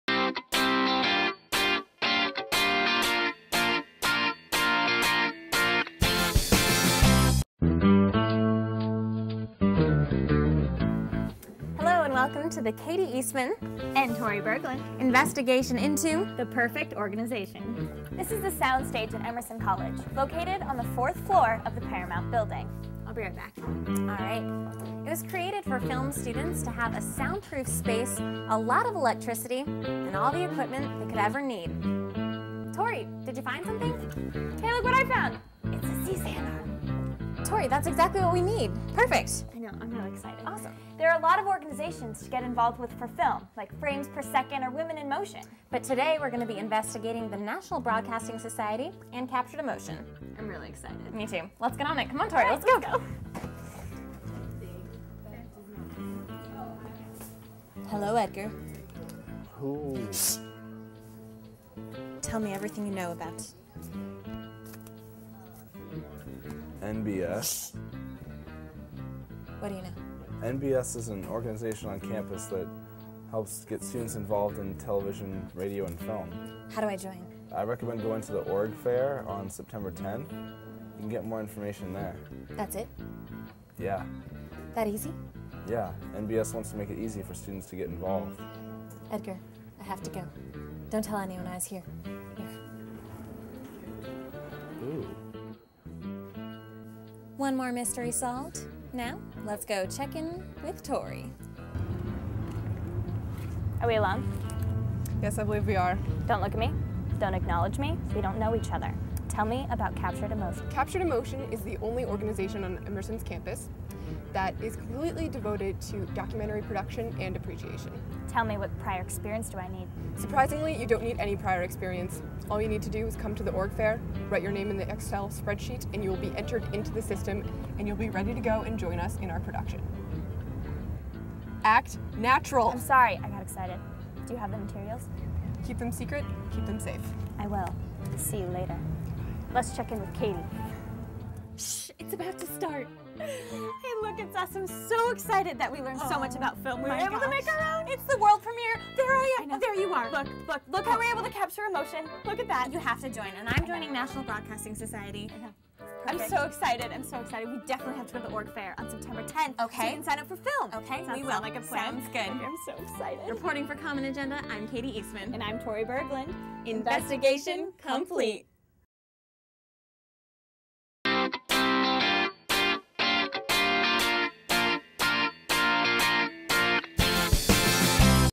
Organization Investigation: Film
Find out about the National Broadcasting Society and Captured Emotion at Emerson College.